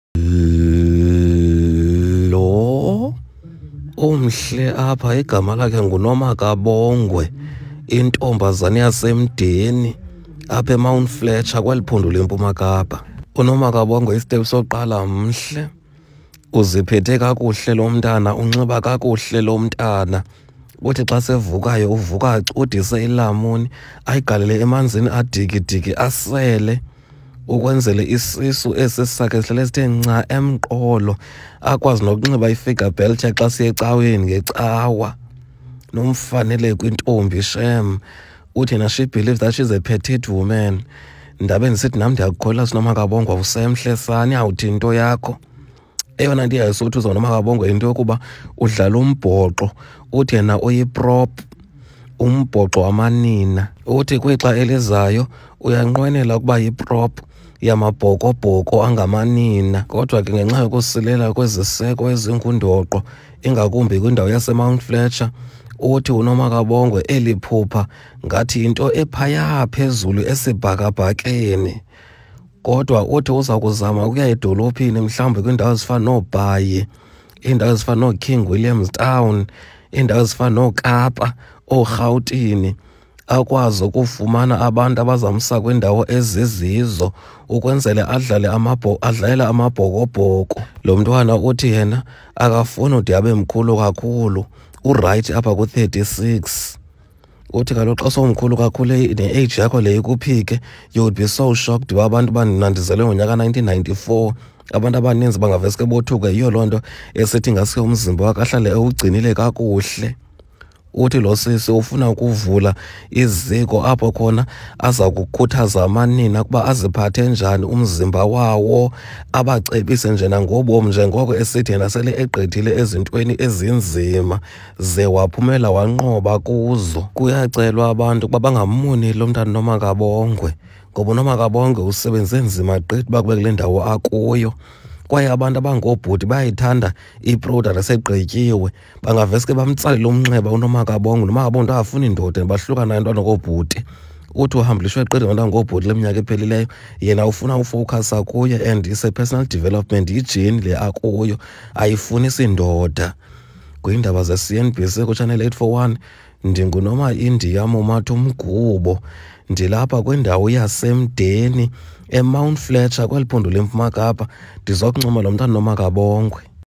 Xhosa Comedy News - uSis Nomakabongwe
uSis Nomakabongwe yintombi yaseMdeni eMount Fletcher kweliphondo leMpuma Kapa. Kuthiwa uwuthanda kakhulu umbhoxo nebhola yomnyazi. Kwaye uzivulele elakhe iziko lokuzilolinga nafuna abantu abangosisi babeyinxalenye yalo 😍🥰💋🔥🔔👌💣🤯😻💃🎵🎶

Intatheli yethu esenzele elinqaku nguNomaIndiya Mumathumgubo 👇
Follow Xhosa Comedy News on FB :